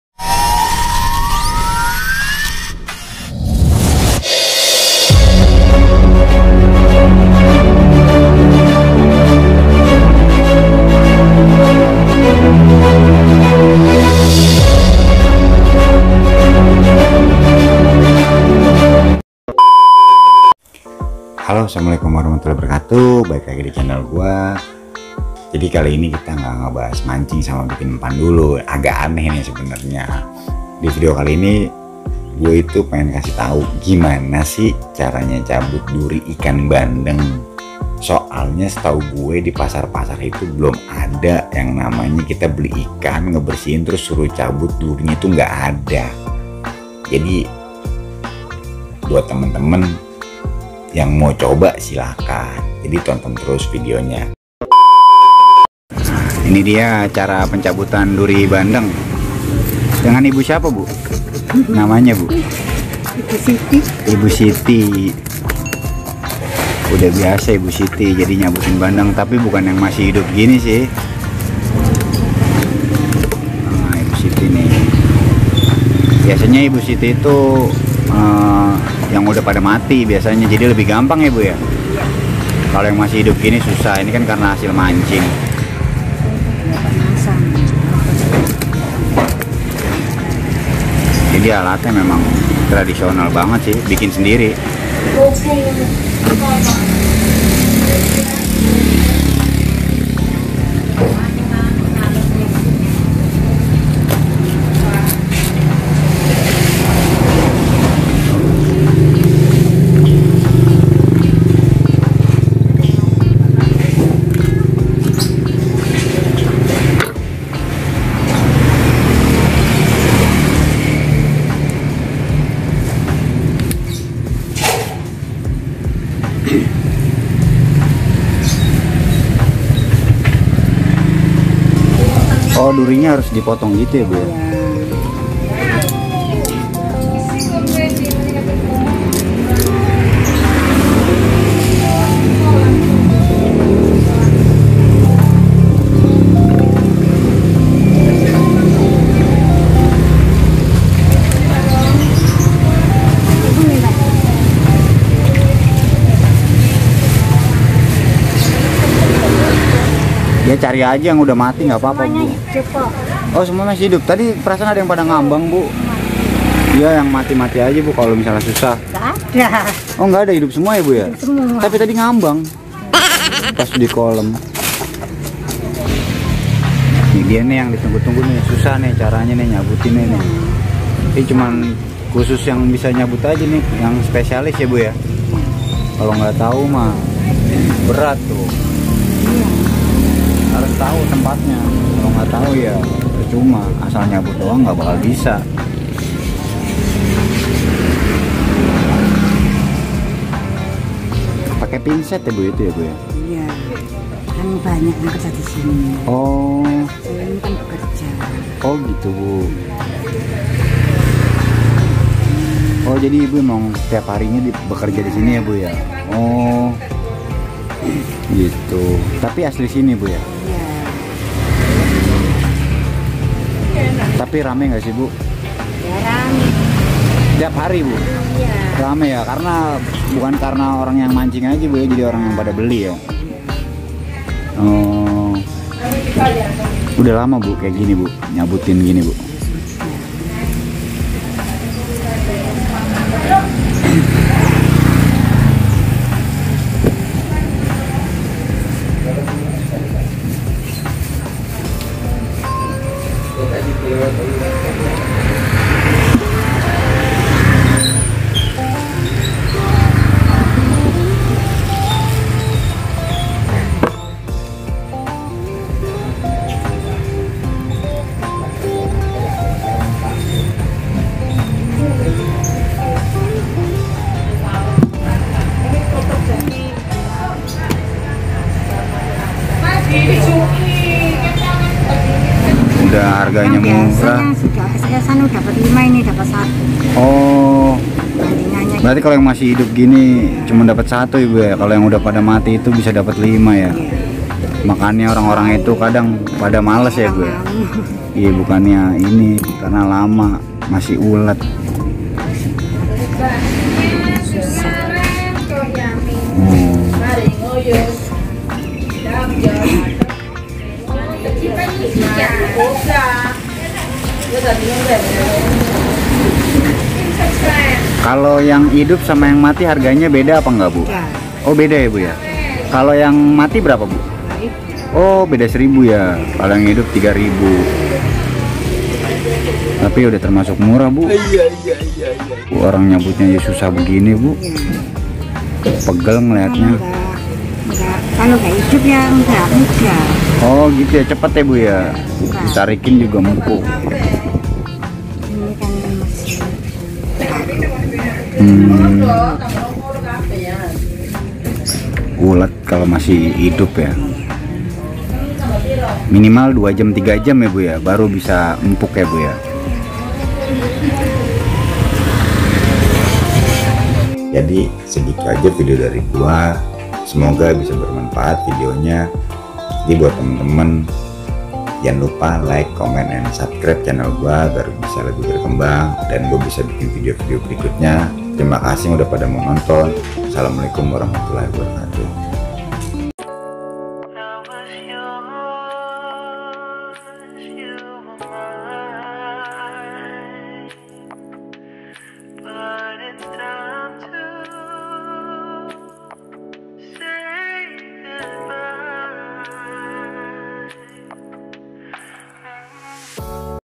CABUT DURI IKAN BANDENG||MURAH CUMA 2000 PERAK
Asalamualikum wr wb

Divideo kali ini kami ingin berbagi tips bagaimana cara mencabut duri ikan bandeng,tonton videonya jangan diskip yah.

Asalamulikum wr wb

Facebook  : hari raise ola ole
Instagram :  HARI RAISE ROD GARAGE